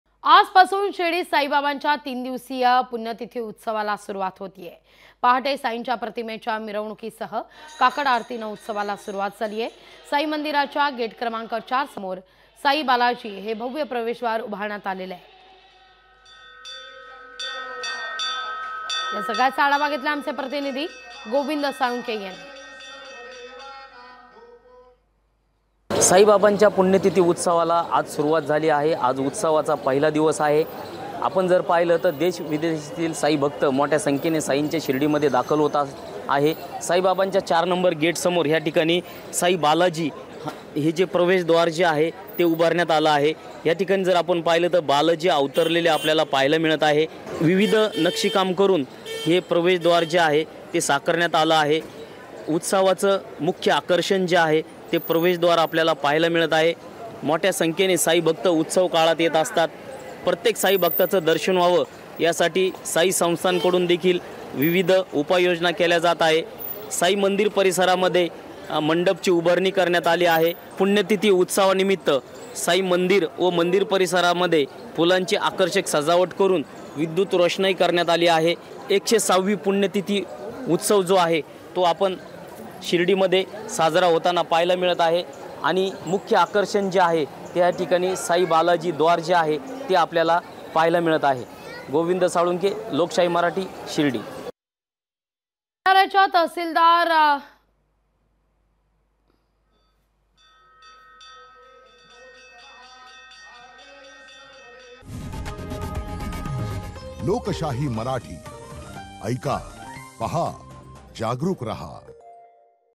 Shirdi | Sai Baba | शिर्डीत साईंच्या पुण्यतिथी सोहळ्याला सुरुवात | Marathi News
Shirdi | Sai Baba | Sai's death anniversary celebrations begin in Shirdi Marathi News